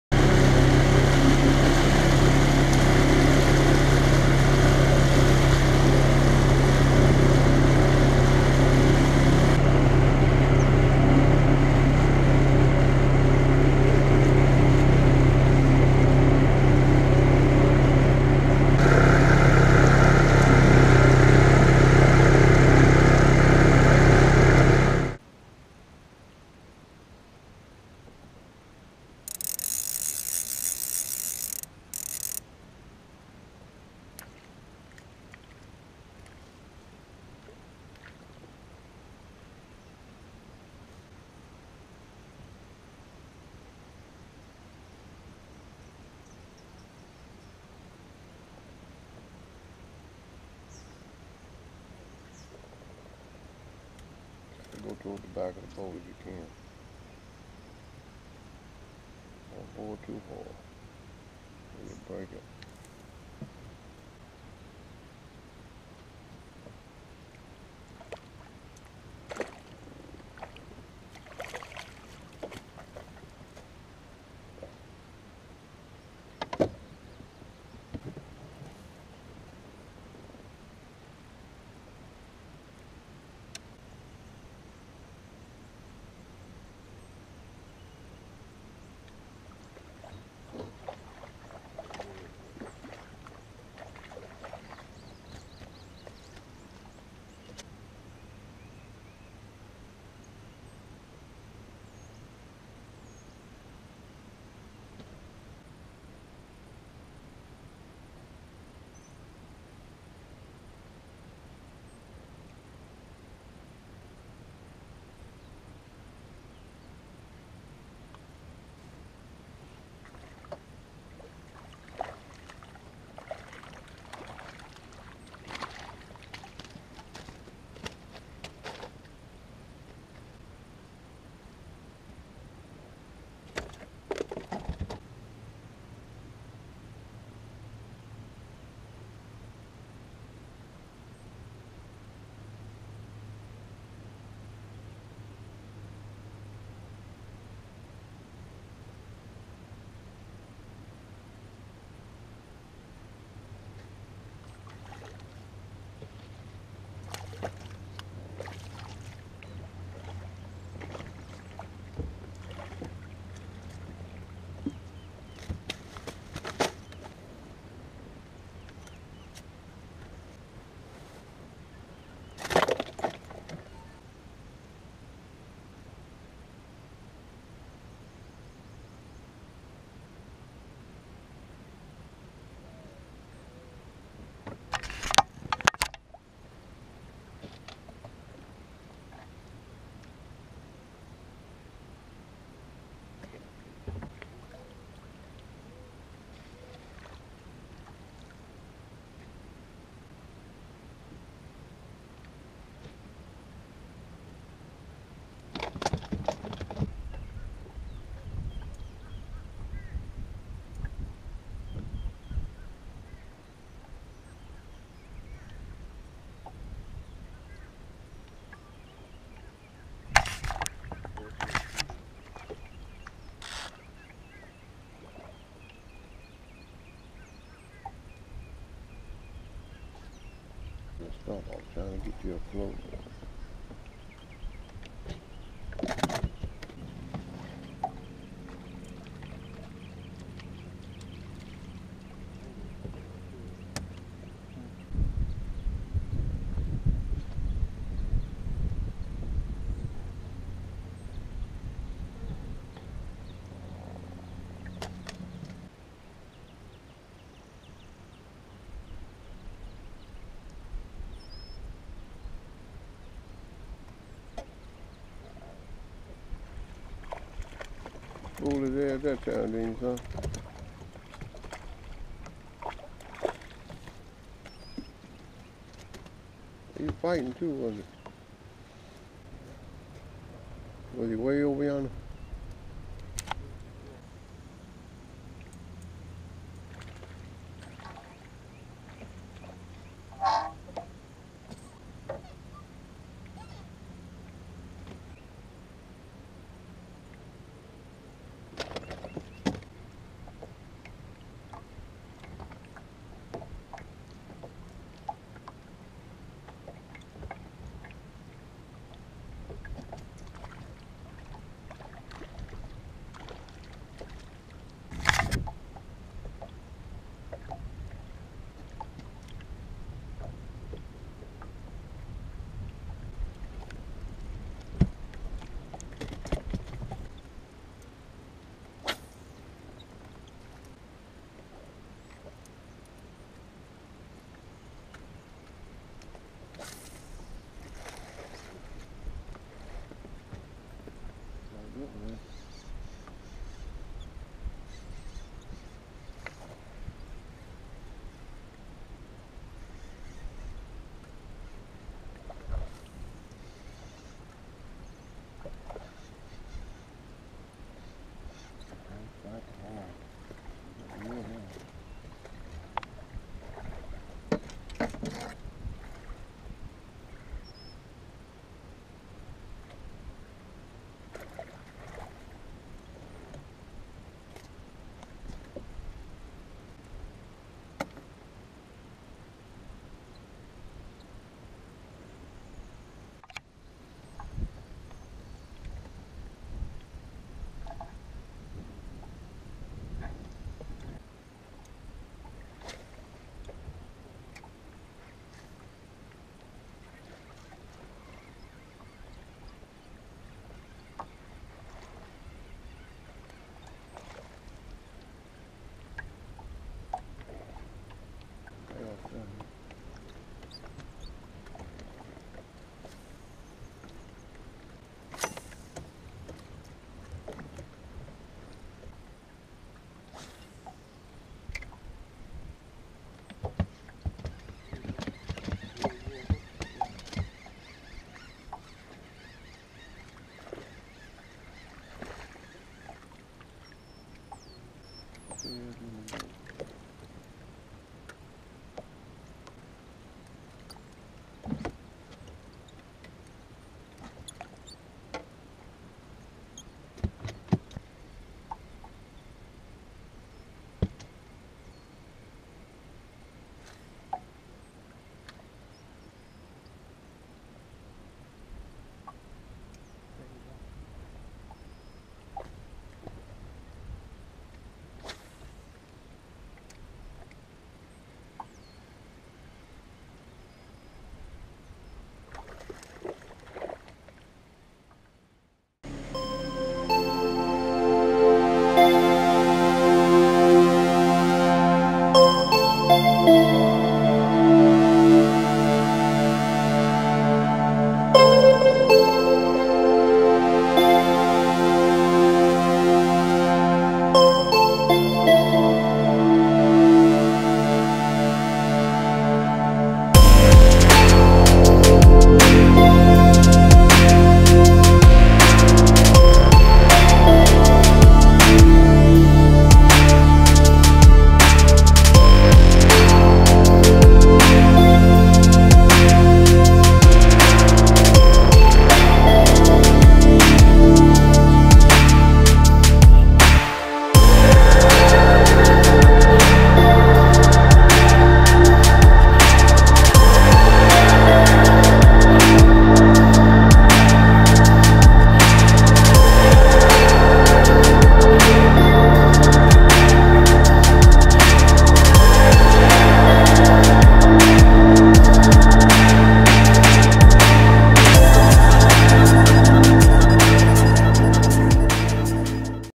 My Personal Best Striped Bass While Crappie FIshing! Season 1 Episode 8
We were crappie fishing and spotted a school of bass!